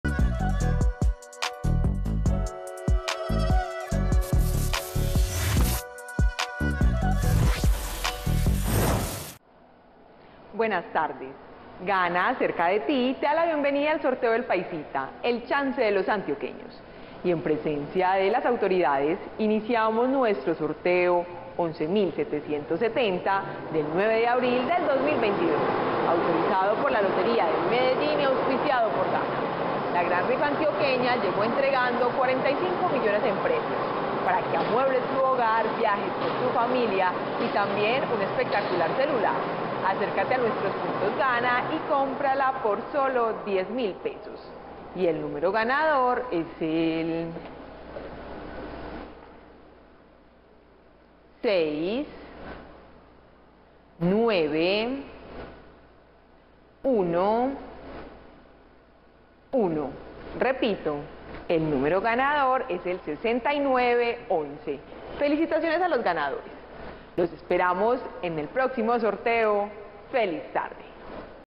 EL PAISITA 2 Resultados del Chance del SÁBADO 09 de abril de 2022 ✅🥇🔥💰
Hola buenas noches, te presentamos los resultados de las loterías y chances en Colombia, vamos con el Resultados del Chance El Paisita 2 del SÁBADO 09 de abril de 2022.

el Resultado Lotería de Boyacá SÁBADO 09 de abril de 2022
Resultado Baloto Revancha SÁBADO 09 de abril de 2022

Resultados Lotería de Boyacá, Lotería del Cauca, Baloto Revancha y sorteo Astro Luna